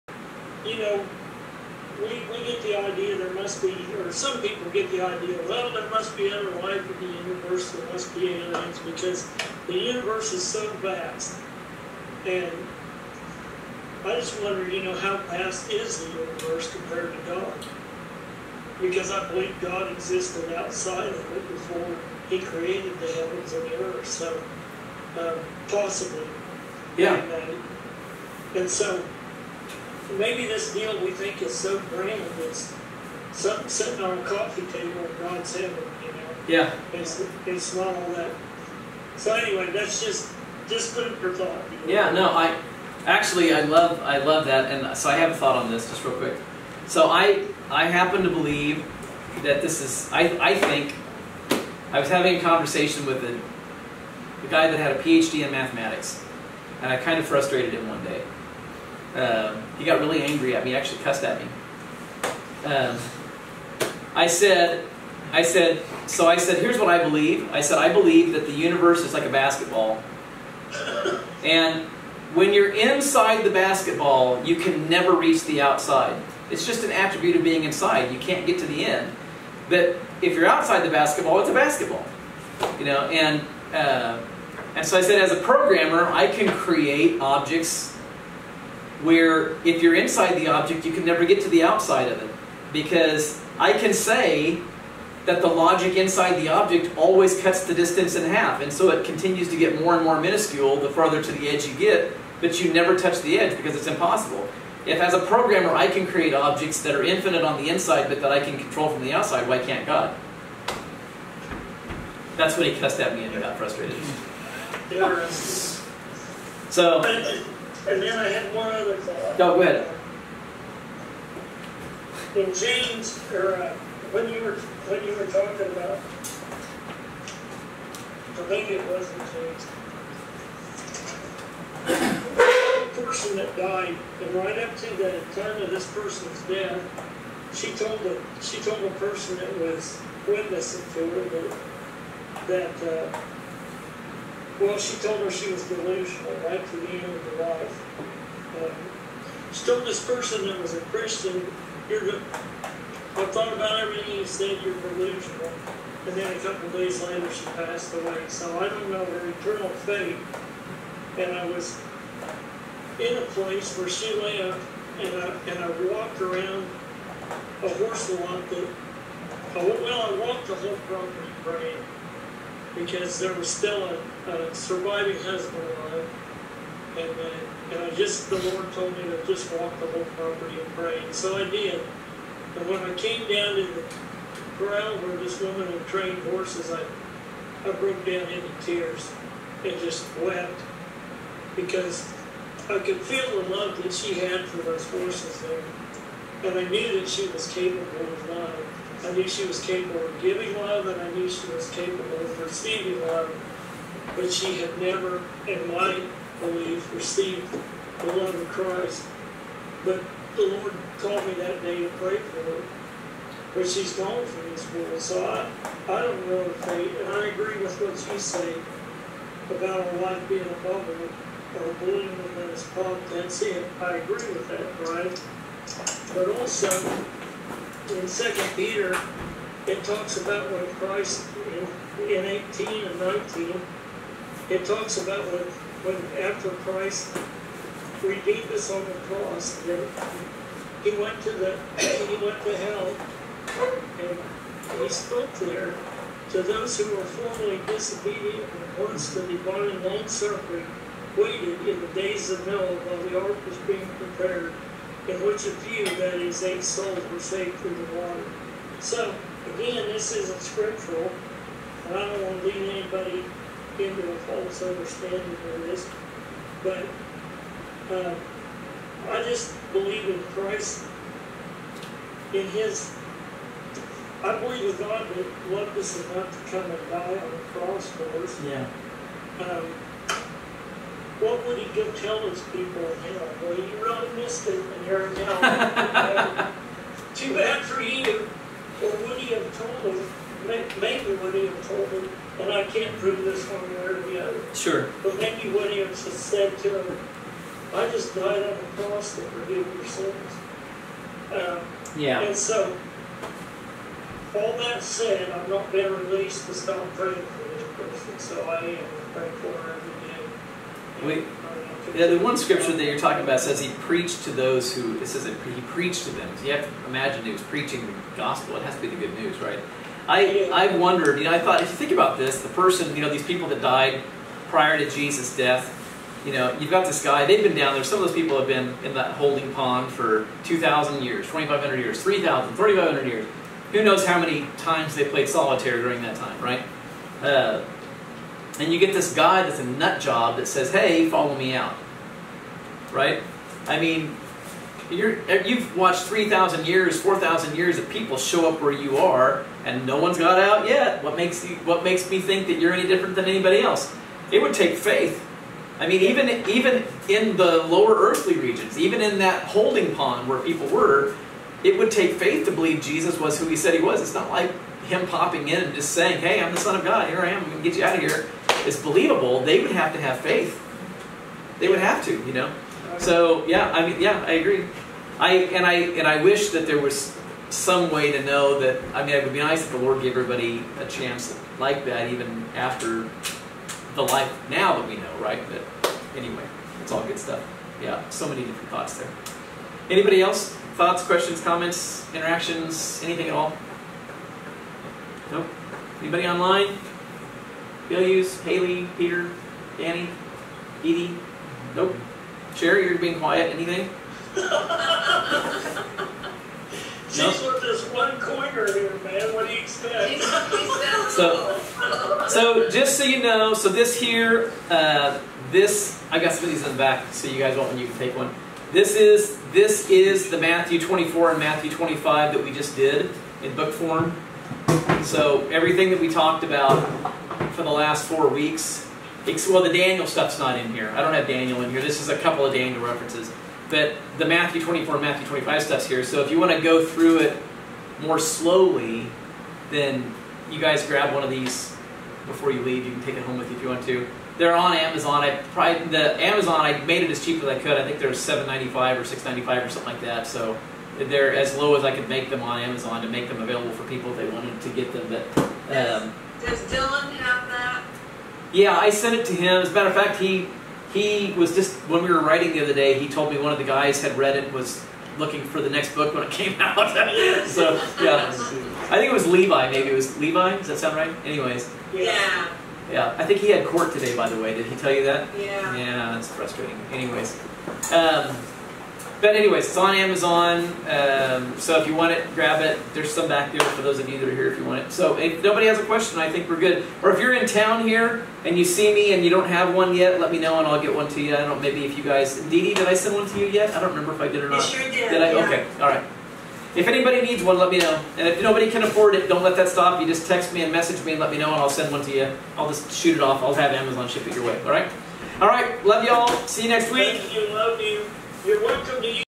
22102814
This video is part of a playlist.  To see the whole teaching go to @buckhurlbut/playlists.  A walkthrough of the books of the Revelation, Daniel 7, Daniel 8, Daniel 9, Daniel 10, Daniel 11, Daniel 12, Matthew 24 and Matthew 25.